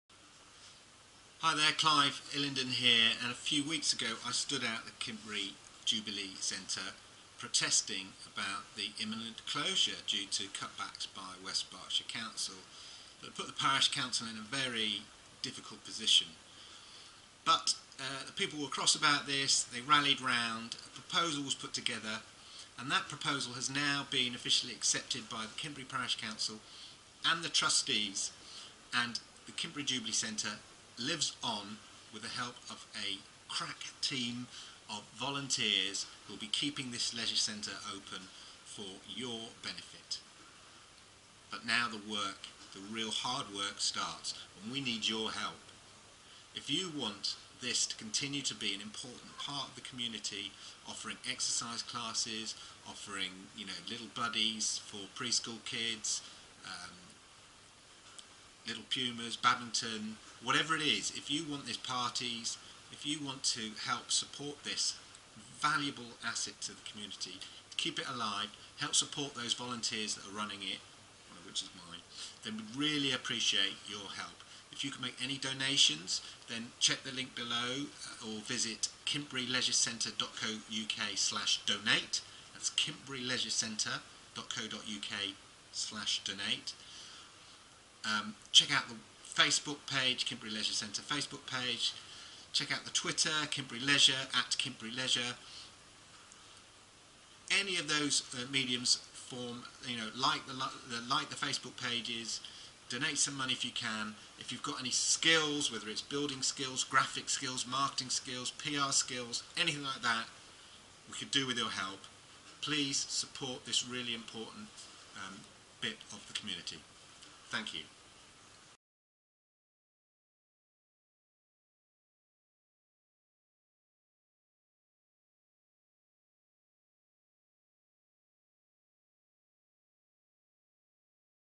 Kintbury Jubilee Centre - Support The Kintbury Leisure Centre
A few weeks ago the Jubilee Centre in Kintbury was facing imminent closure. (you may have seen my video). Well, good news! A proposal to keep the Kintbury Jubilee Centre open was put to Kintbury Parish Council (the trustees). This proposal has been accepted, and a team of volunteers will now run the centre for the community. But we NEED your help. More details at